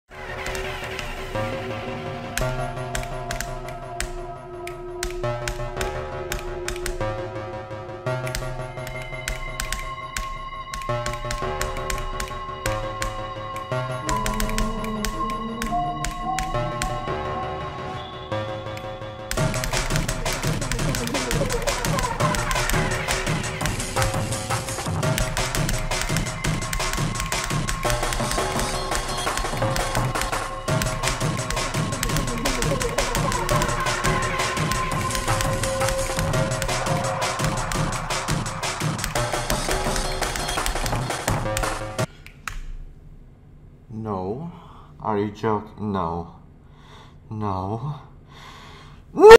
Last jump…
私はこのレベルが嫌いなので、自殺したいです
i died to 80s today Yes today over 50 times.
im pretty sure i will be the unluckiest victor of this level.